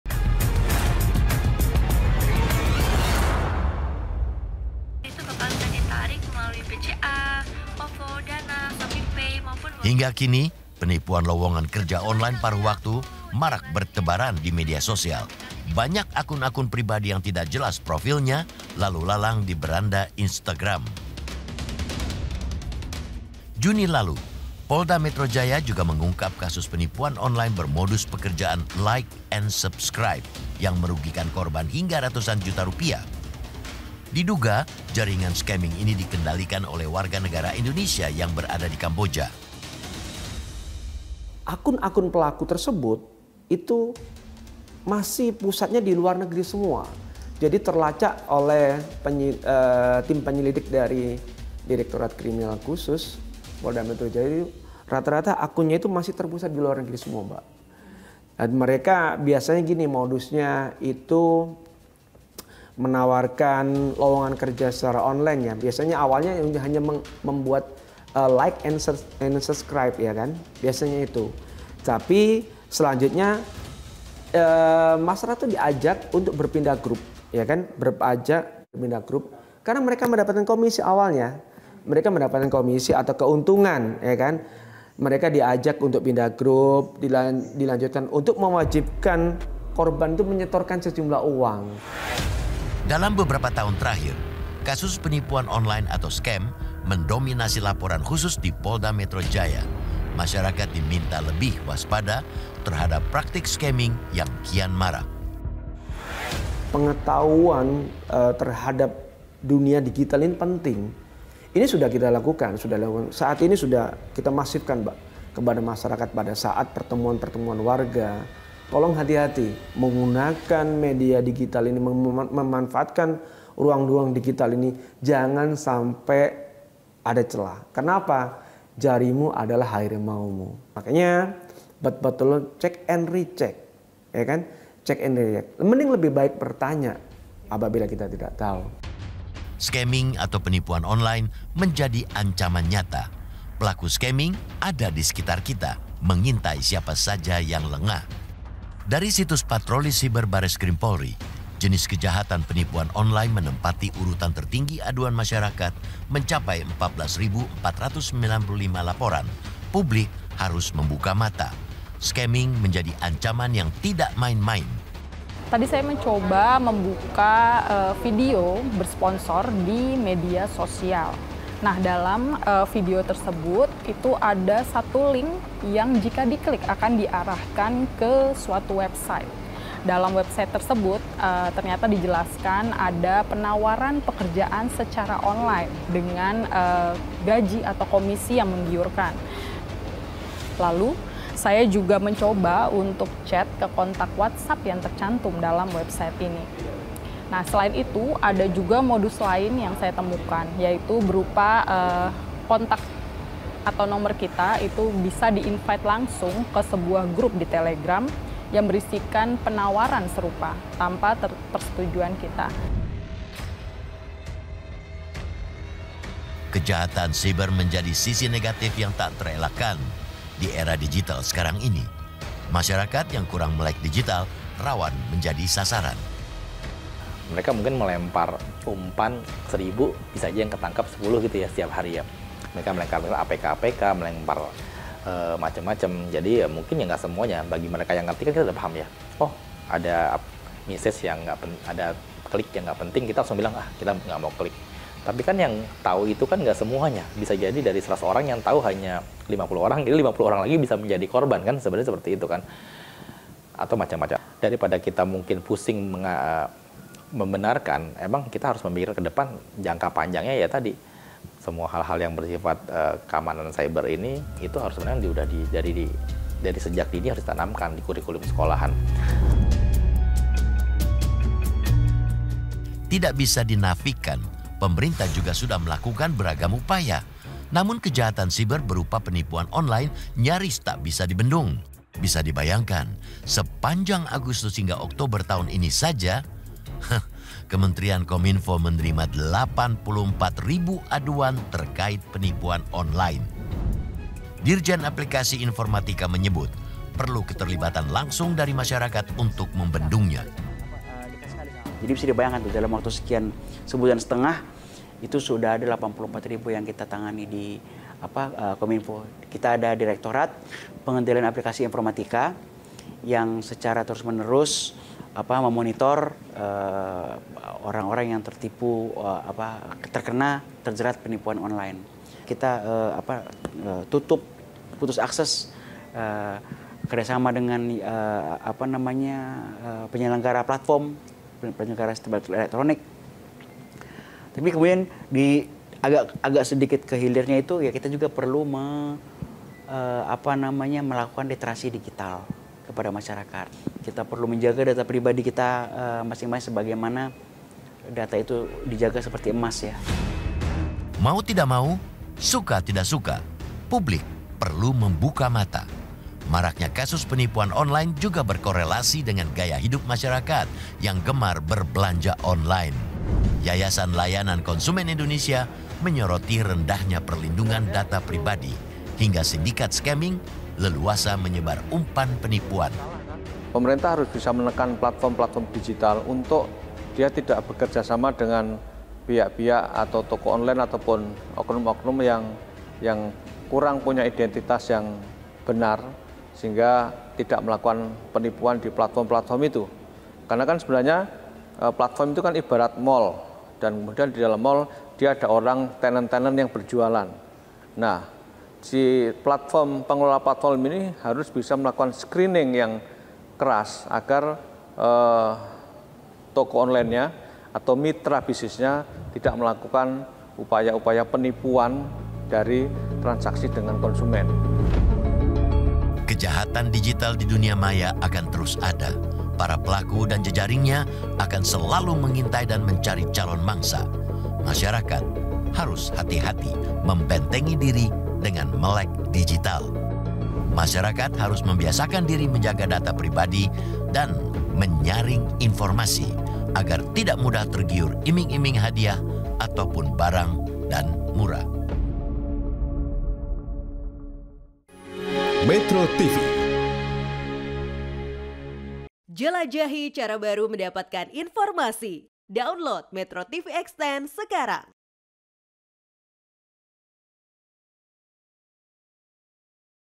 Marak Terjadi Kasus Penipuan Pekerjaan Online
Fakta ini menjadi celah terus bermunculn kasus-kasus penipuan online di indonesia.